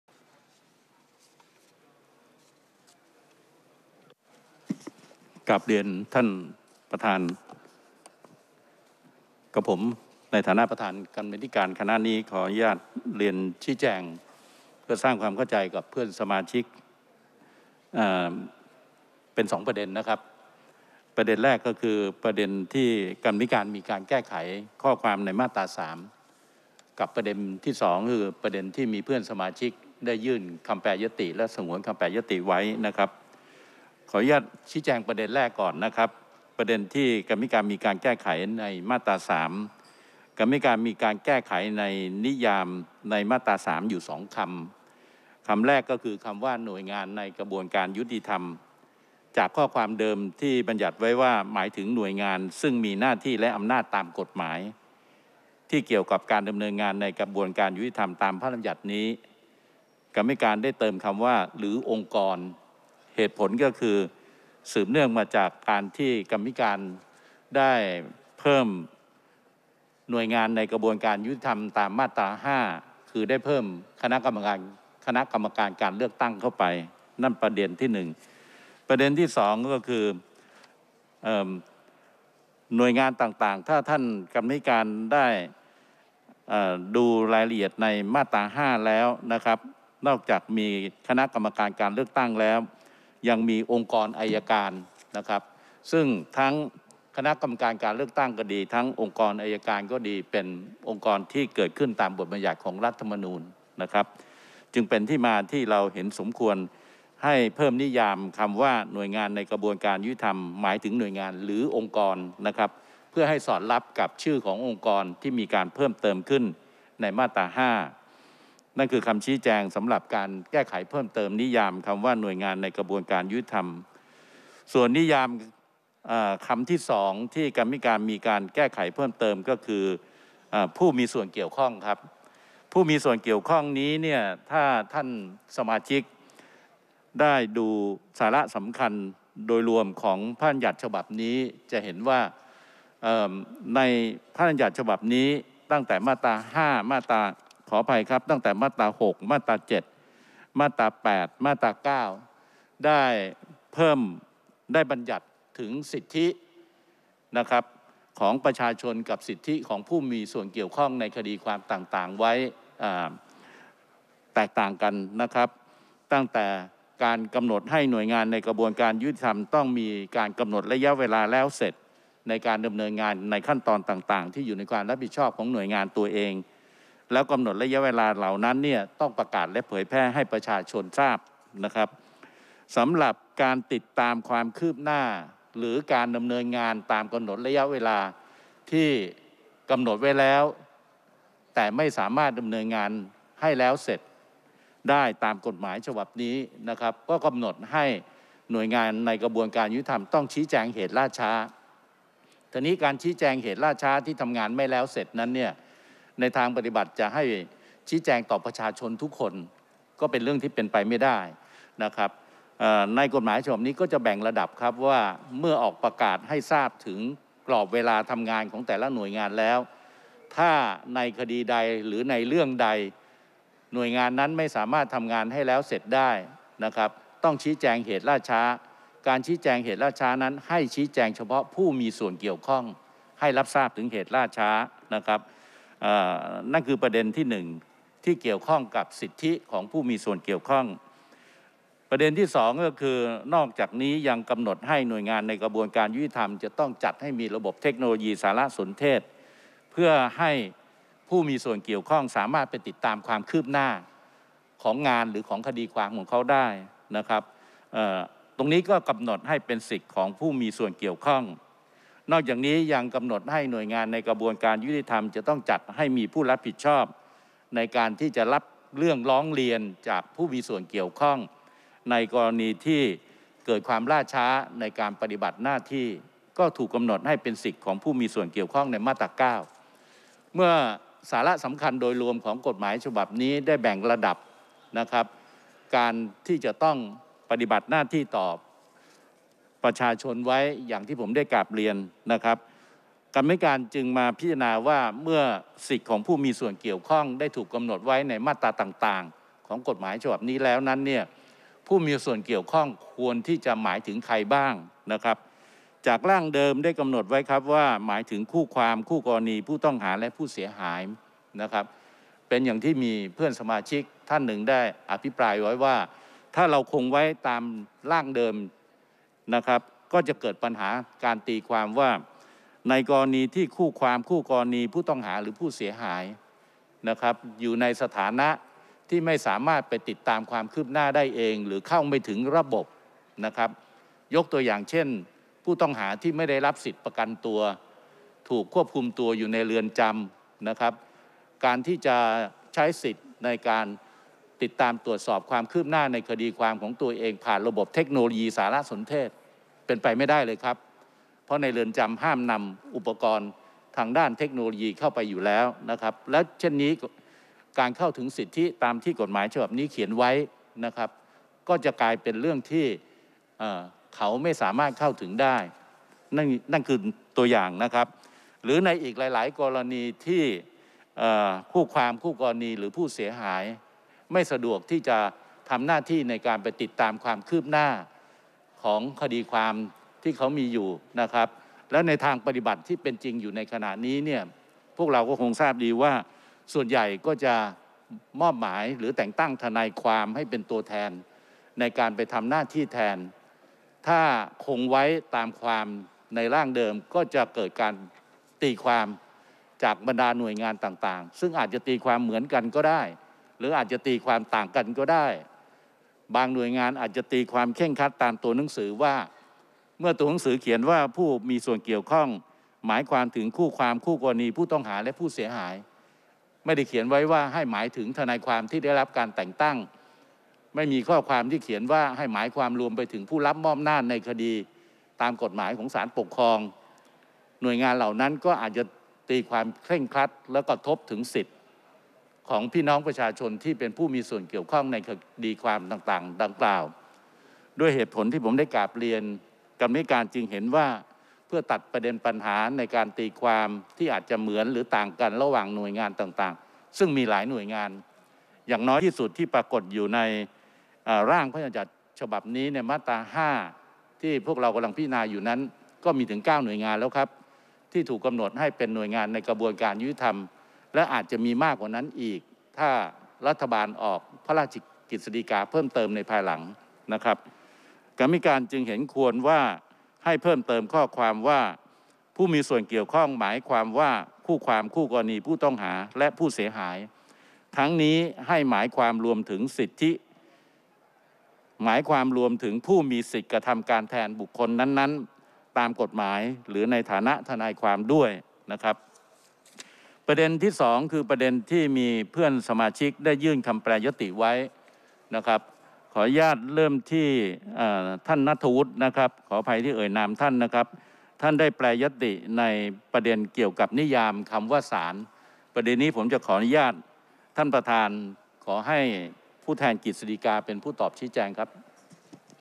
สว.สุรชัย เลี้ยงบุญเลิศชัย คลิปที่สอง อภิปรายร่างพระราชบัญญัติกำหนดระยะเวลาดำเนินงานในกระบวนการฯ
๓.๒ ร่างพระราชบัญญัติกำหนดระยะเวลาดำเนินงานในกระบวนการยุติธรรม พ.ศ.
ซึ่งคณะกรรมาธิการวิสามัญพิจารณาเสร็จแล้ว
(ค้างมาจากการประชุมร่วมกันของรัฐสภา ครั้งที่ ๙ (สมัยสามัญประจำปีครั้งที่หนึ่ง) วันอังคารที่ ๒๖ กรกฎาคม ๒๕๖๕)